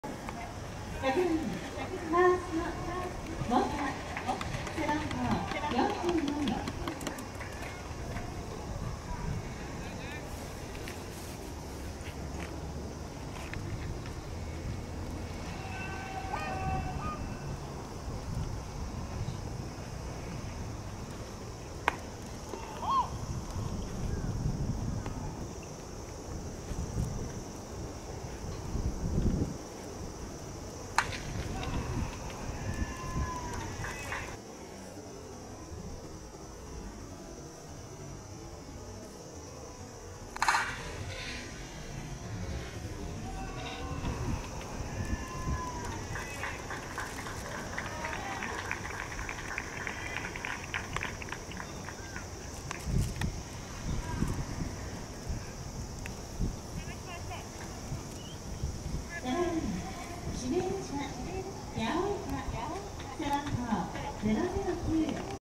力の差を見せつける！二軍戦で強烈な打球をセンターに弾き返すモタ！
二軍戦で強烈なセンター前ヒット！
早く一軍で見たいモタのバッティング！